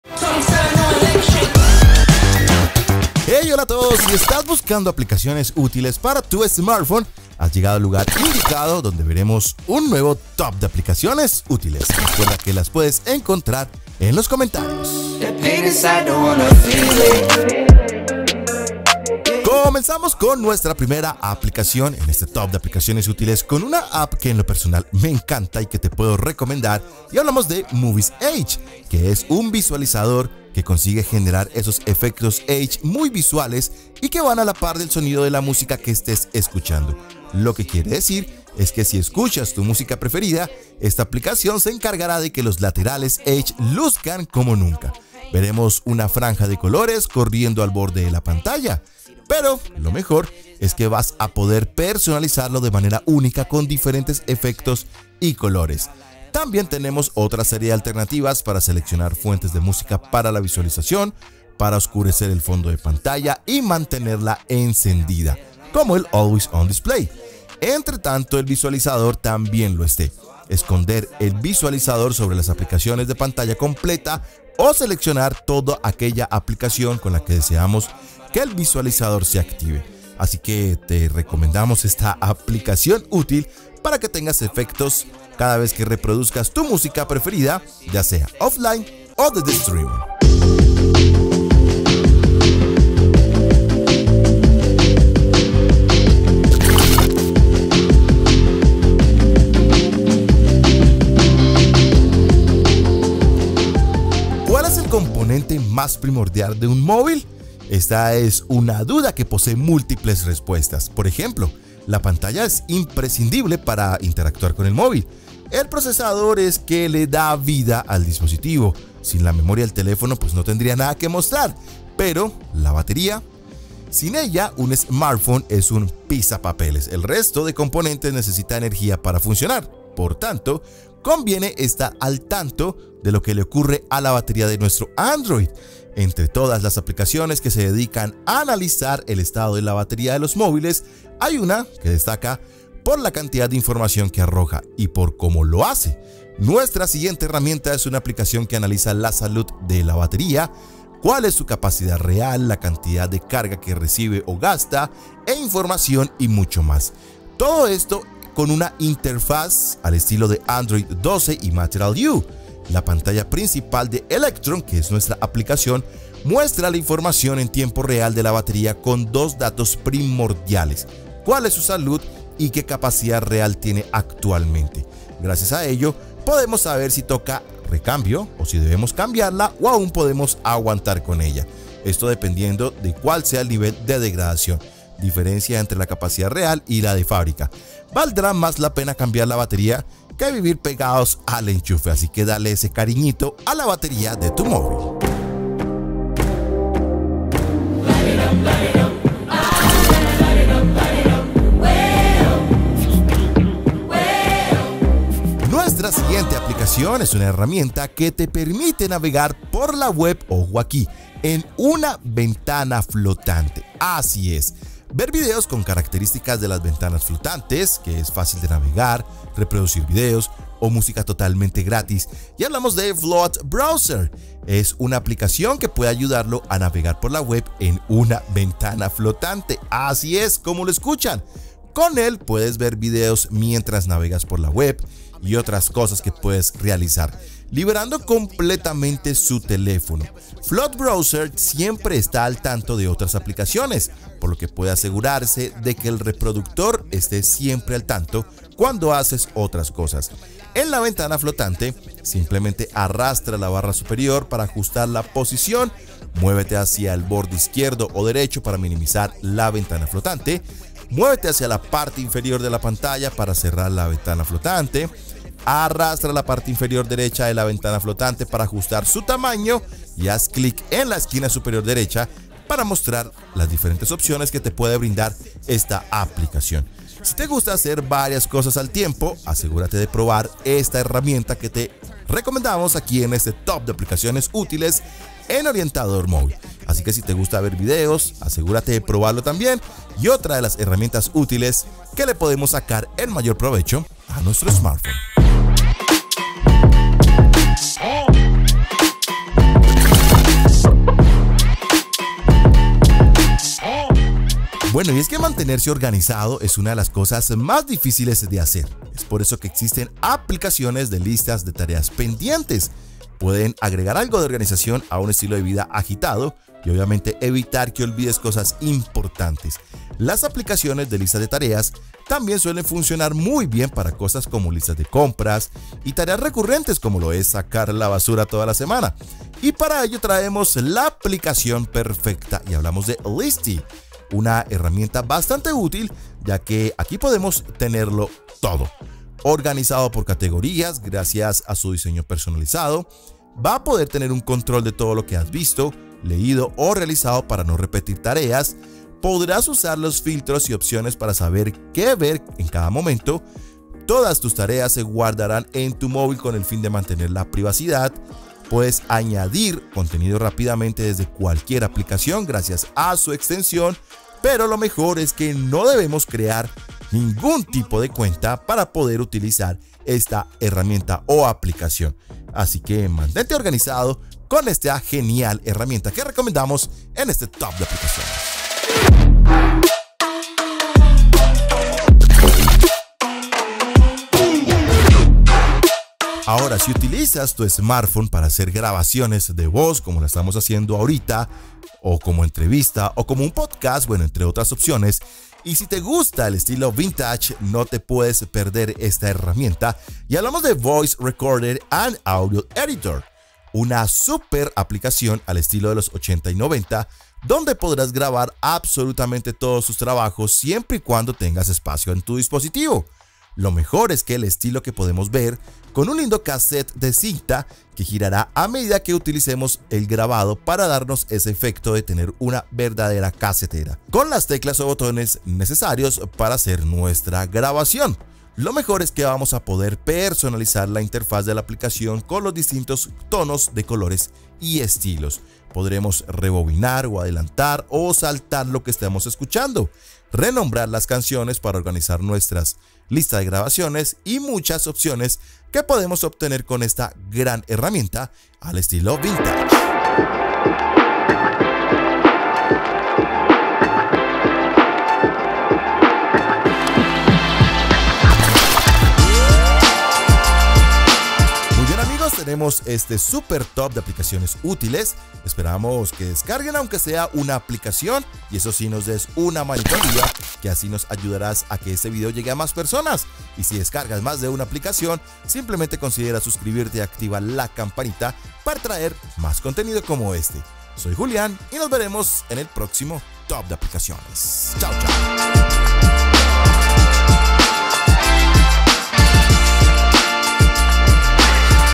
🤖 Aplicaciones ÚTILES Para Tu ANDROID 🔥 [TOP de Apps] 👉 2024
📍IMPORTANTE: Este video es informativo, en ningún momento se Infringe los lineamientos de la comunidad de YouTube.

Te traemos un TOP con las mejores aplicaciones útiles para  tu móvil Android.

Videos de tu Interés

Top de Apps

📍00:00 Intro
📍00:16 Muviz Edge
📍01:36 Electron
📍03:19 Float Browser
📍04:59 Listy
📍06:21 Voice Recorder
📍07:37 Final